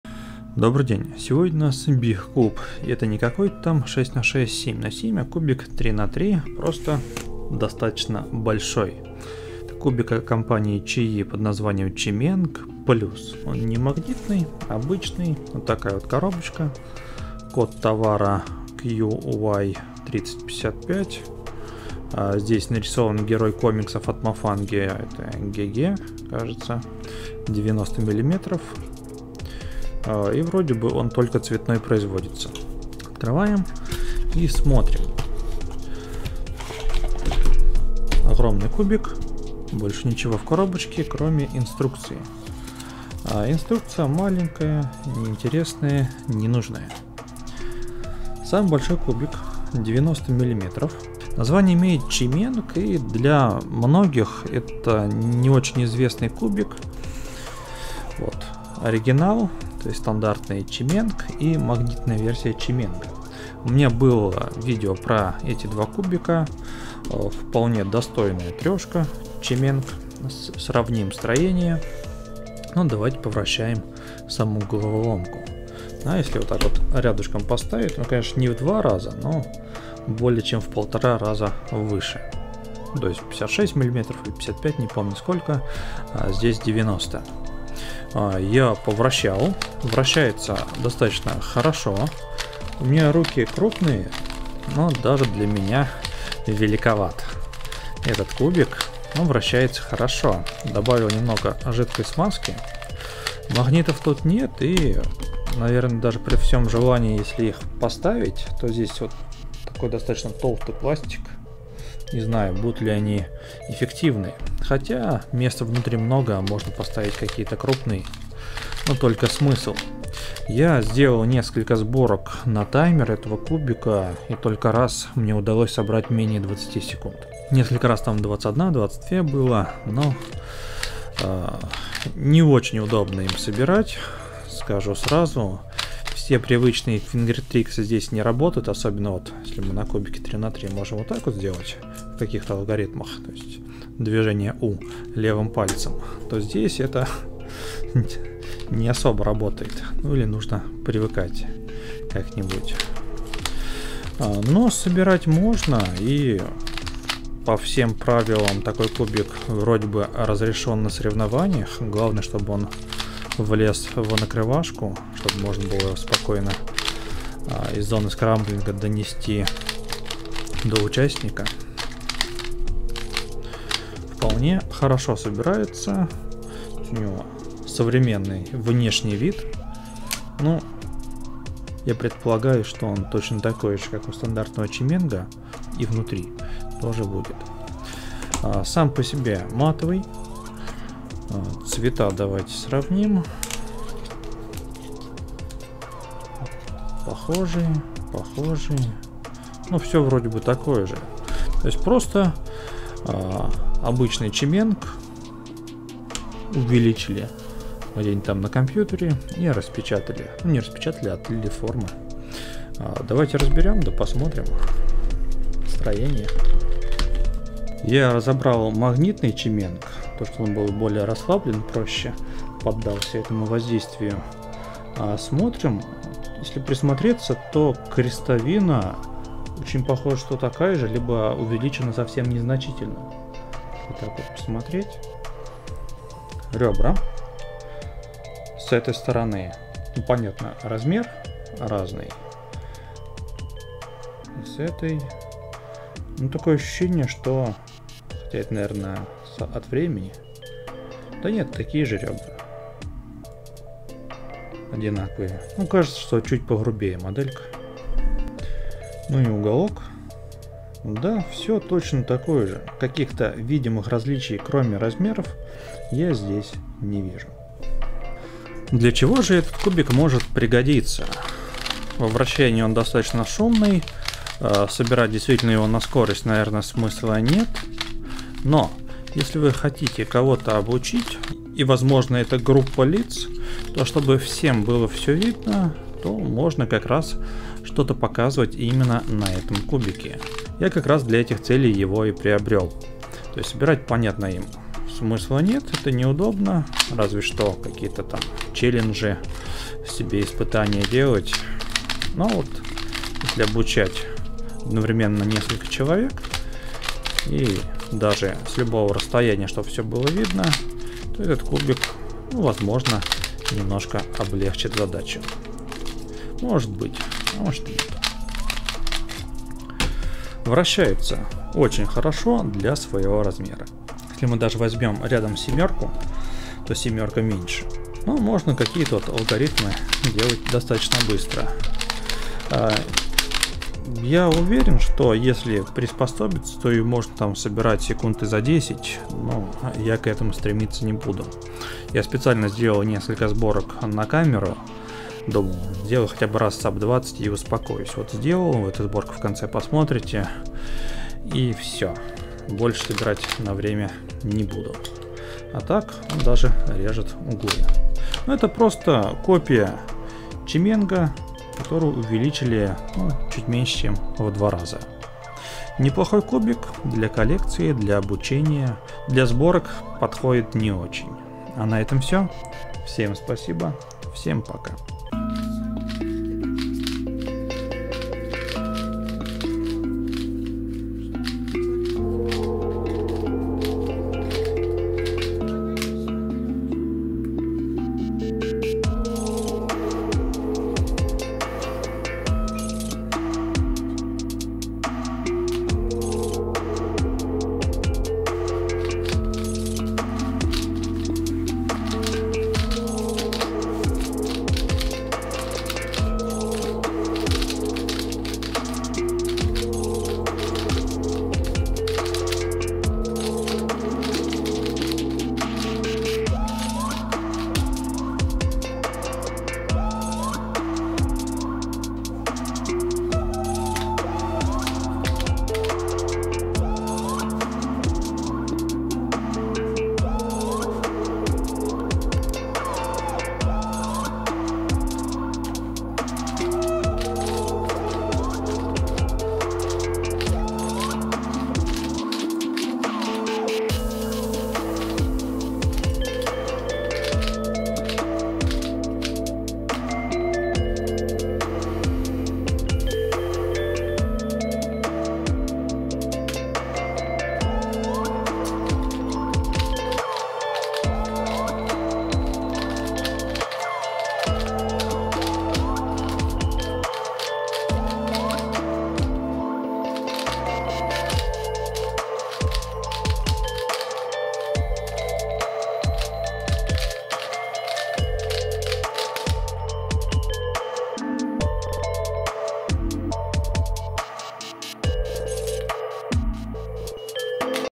Unboxing №235 QiYi QiMeng Plus 3x3 90 mm
Кубик Рубика 3х3 увеличенного размера. Собирать на скорость можно, но лучше использовать для чего-то другого.